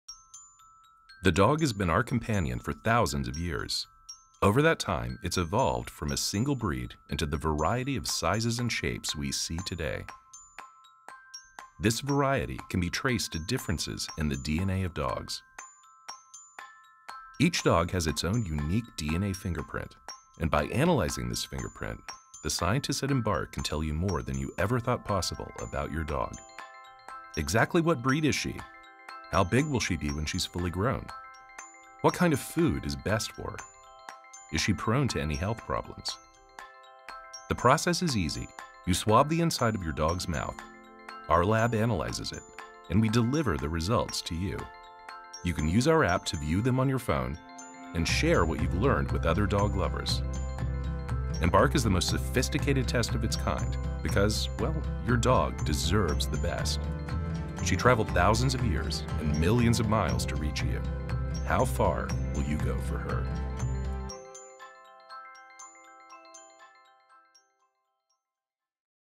Embark Dog DNA Test - Get to Know Us
Embark's dog DNA test will identify your pup's breed, health needs, and ancestry.